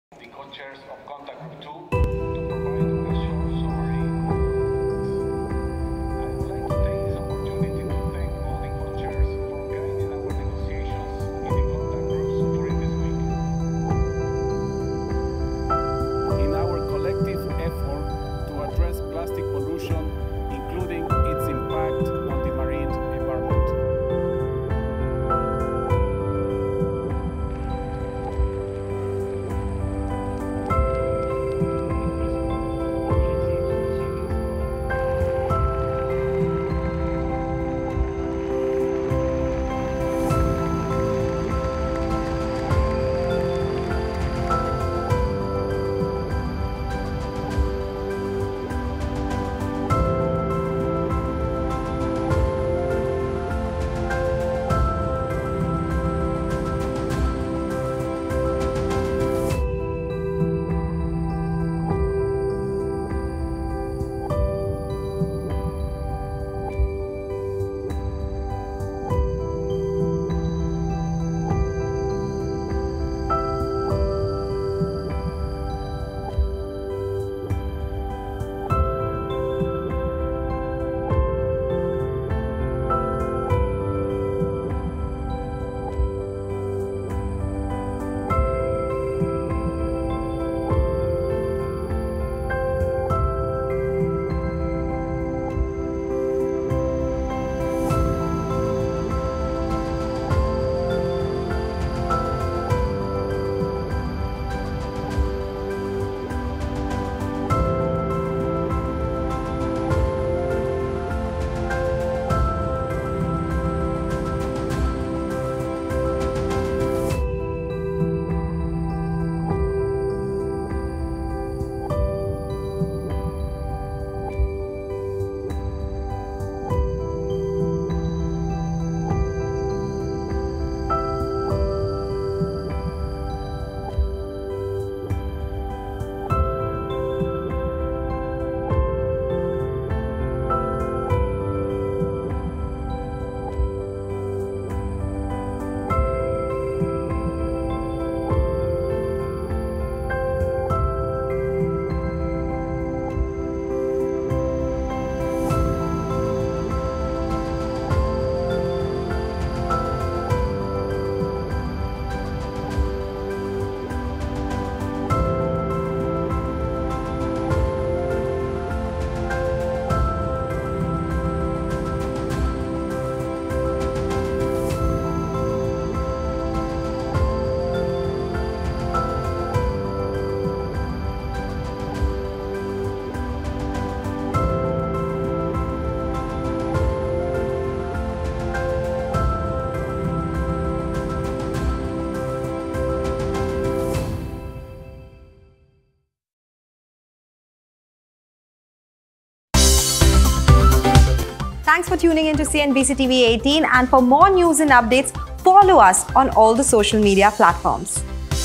Countries Fail To Reach Consensus On UN Plastic Treaty | INC-5 Meeting In Busan | N18G | CNBC TV18
Plastic Pollution Treaty Talks Collapse!
Countries negotiating a global treaty to curb plastic pollution failed to reach agreement. Negotiators will meet again next year to finish the talks.

n18oc_world